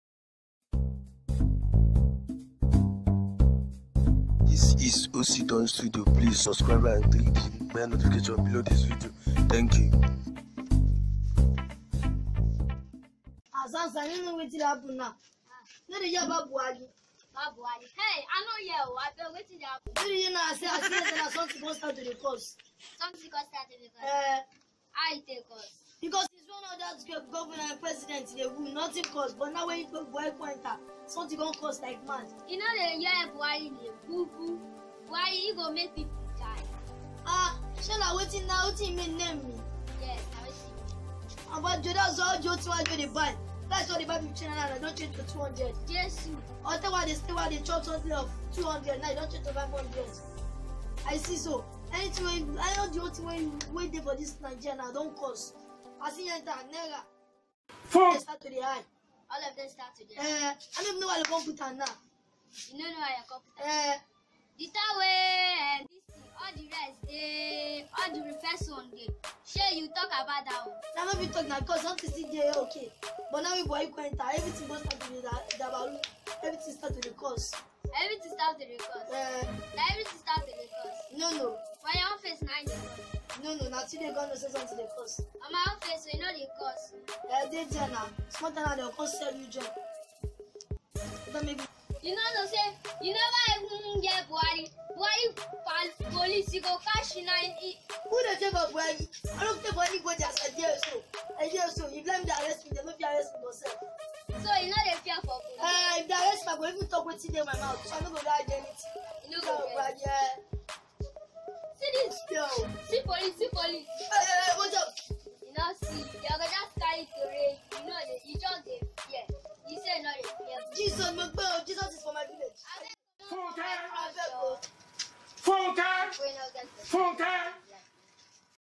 BUHARI'S TIME OCY DUN STUDIO EPOSODE 341
Buhari's Time shows where  this guy and Sansa were argument about Buhari's Leaderahip with Gozie, always watch our video on YouTube and